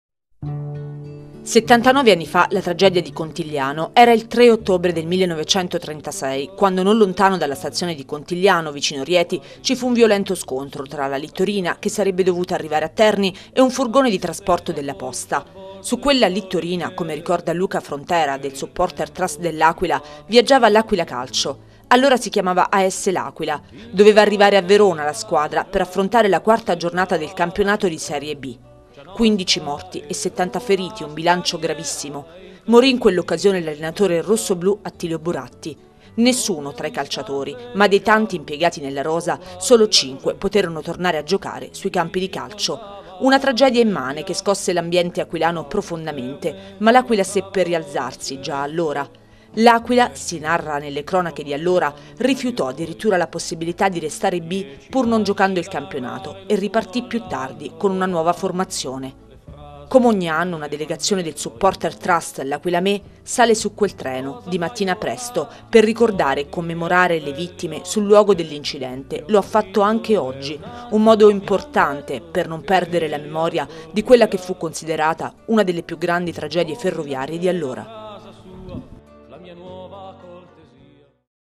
79 anni fa la tragedia di Contigliano
Ricorre oggi il 79esimo anniversario dell'incidente ferroviario di Contigliano. Sulla littorina coinvolta nel disastro viaggiava l' As L'Aquila Calcio in trasferta verso Verona. Il Supporters Trust L' Aquila me oggi a Contigliano per ricordare, come ogni anno